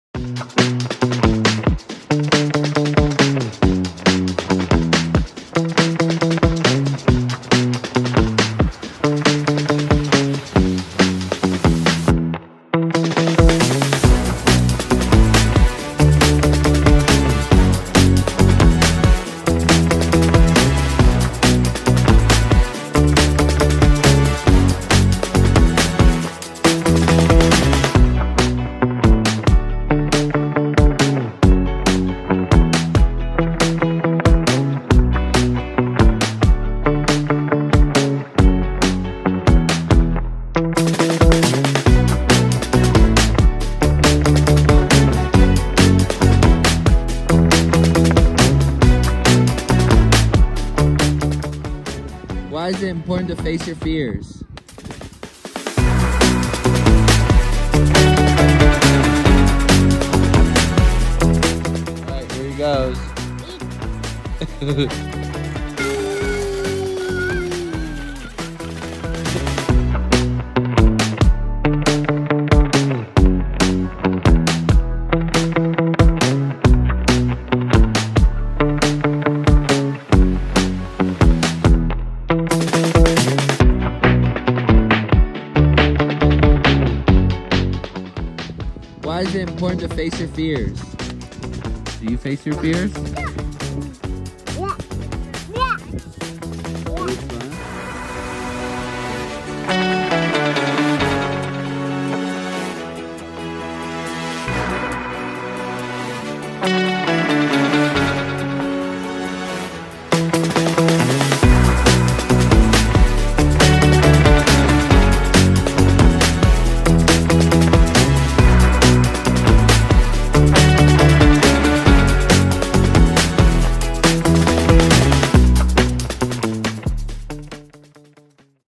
GoPro Fun Part 2 - Lake Tahoe
Just a GoPro edit of some clips from Tahoe! Let me know what you think!
Rock!